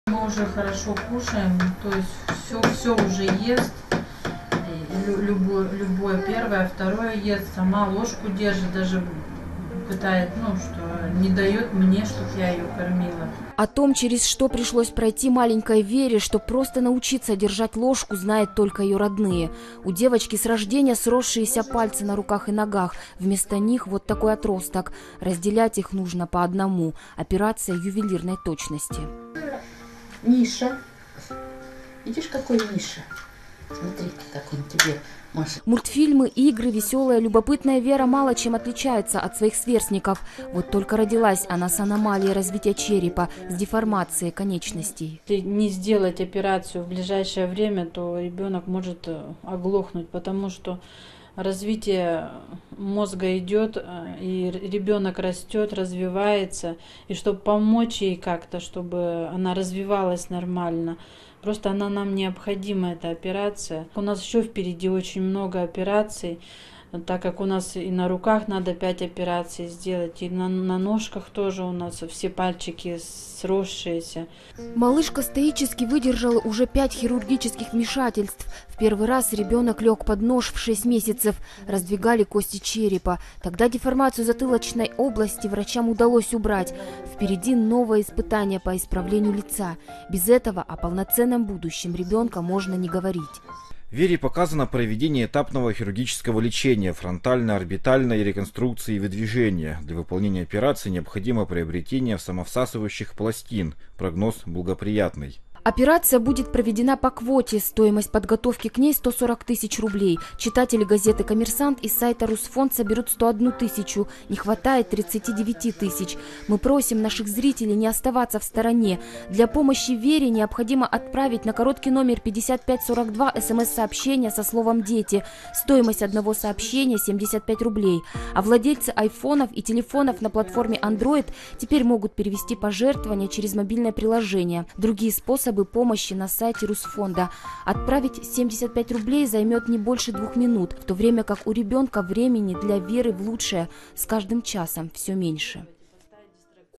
Вера Букина, 2 года, синдром Апера, требуется подготовка к операции
Вера Букина, 2 года, синдром Апера (врожденная аномалия развития черепа, сочетающаяся с деформацией конечностей), требуется подготовка к операции